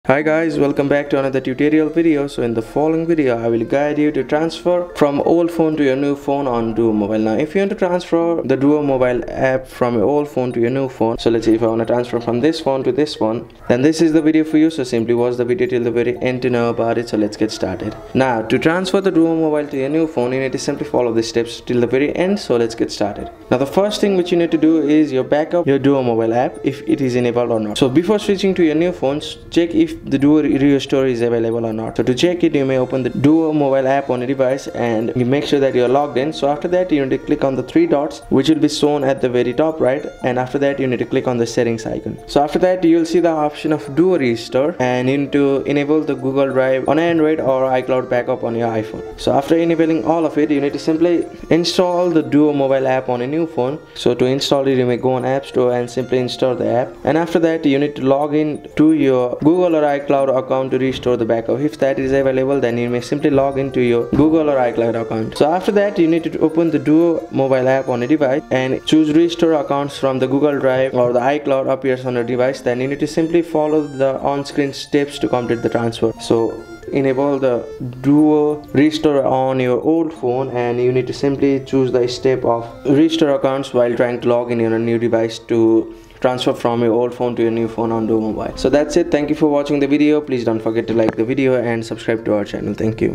How to Transfer Duo Mobile From Old Phone to a New Phone – Step-by-Step Guide - 2025
Want to know How To Move Duo Mobile Without Losing Access but don't know how?
This quick video will guide you to Move Duo Mobile Without Losing Access

So, in this few simple steps you can successfully Move Duo Mobile Without Losing Access

Timestamps :
00:
00:
00:
00: